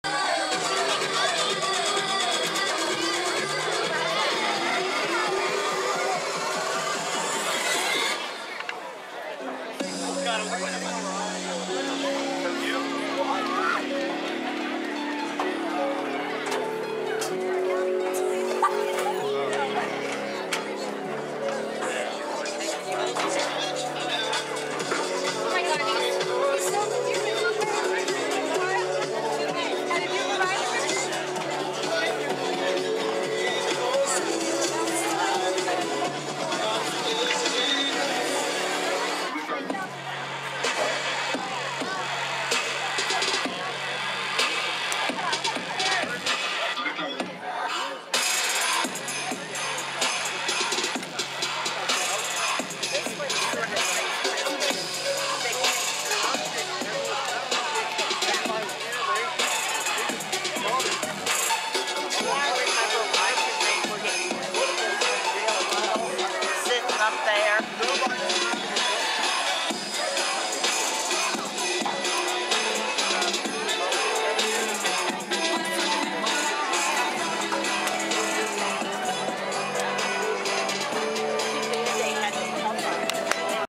Smithsburg vs SCHS Boys Varsity Soccer 10-16-2014 Part 7
Senior Night Pictures - Hard to see because they're covered in plastic due to the rain.
Smithsburg High School @ South Carroll High School
Boys Varsity Soccer
10/16/2014
Smithsburg 0, SC 5